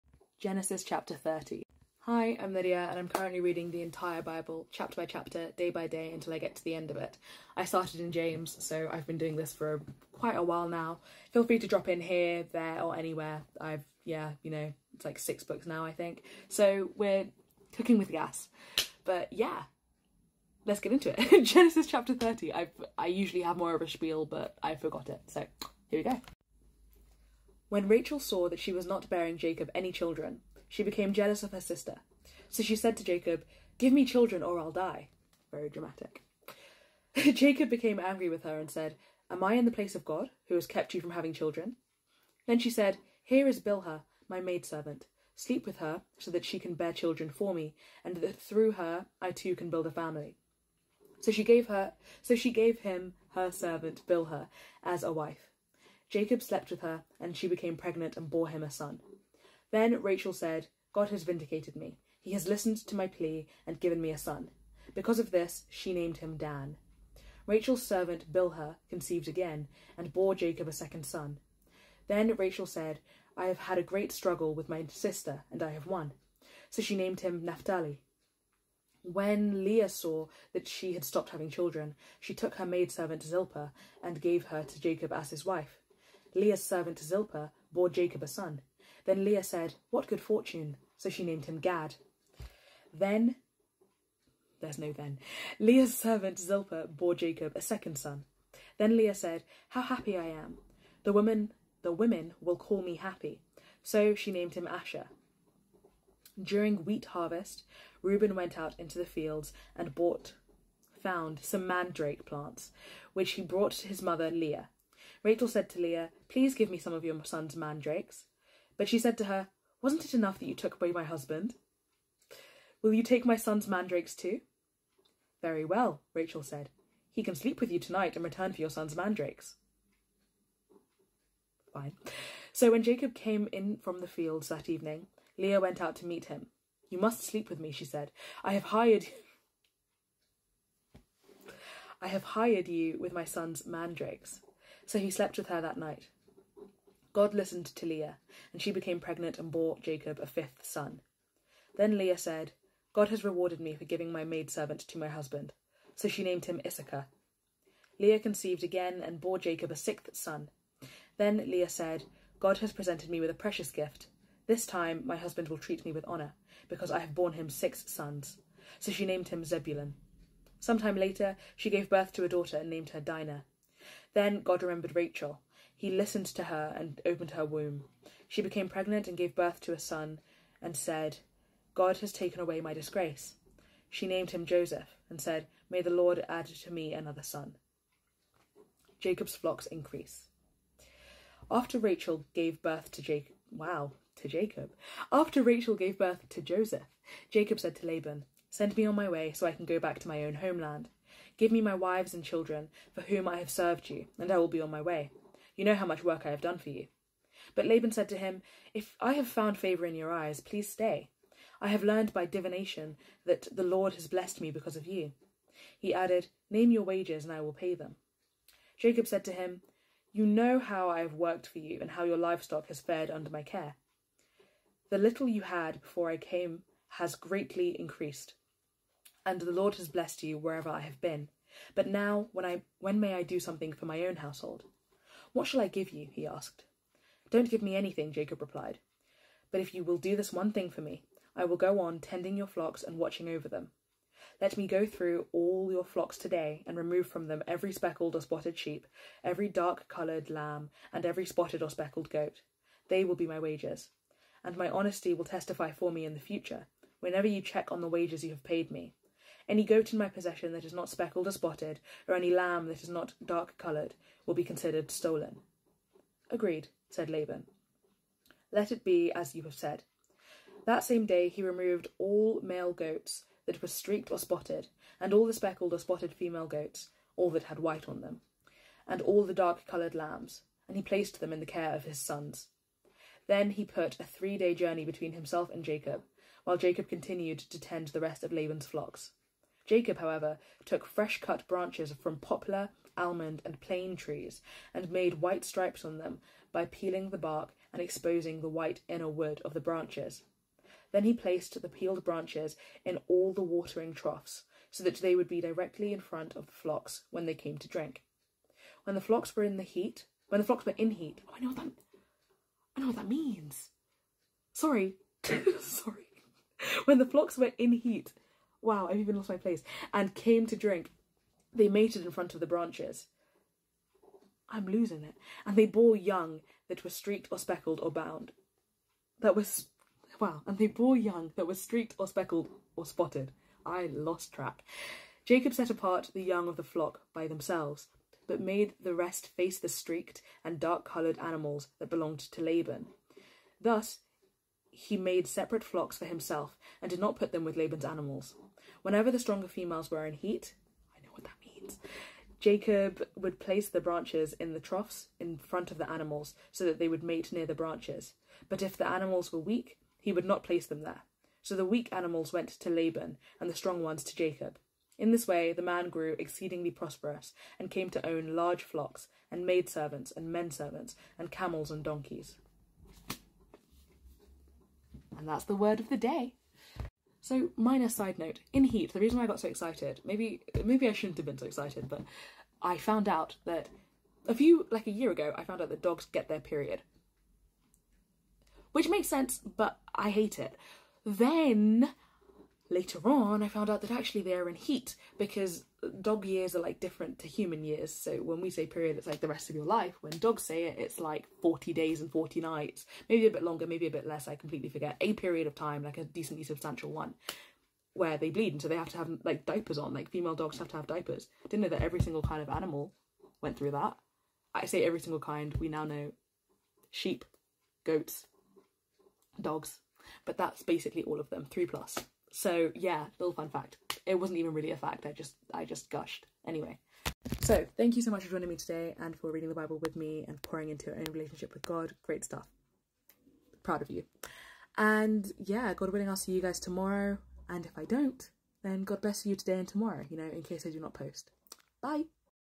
Read a chapter of the bible with me everyday! Genesis 30
Genesis 30 (NIV)

Read the bible with me today :)

English IS my first language so this should be a lot quicker than the other 2 vids ahaha :))

K byeeeeeeeee! 1 Thessalonians 5.23